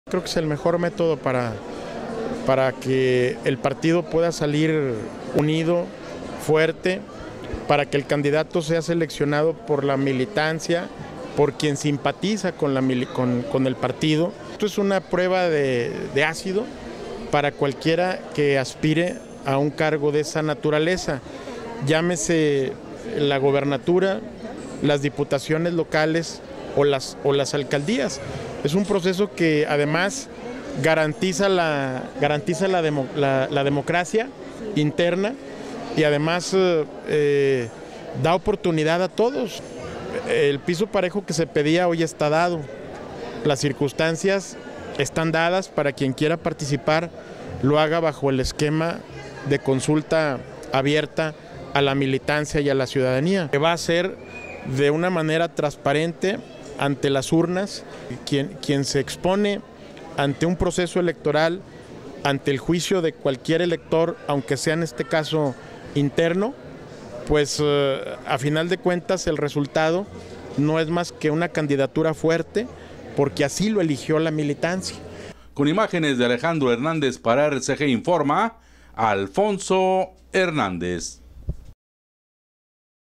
Riquelme en favor de consulta abierta
Torreón, Coah; El alcalde de Torreón, Miguel Ángel Riquelme Solís, consideró que una consulta abierta es el mejor método para seleccionar al candidato del PRI para la gubernatura der Coahuila, además de garantizar la democracia interna en el partido.
Dijo que ese proceso será la “prueba de ácido” para cualquier aspirante a un cargo de elección;  reiteró que una vez que aparezca la convocatoria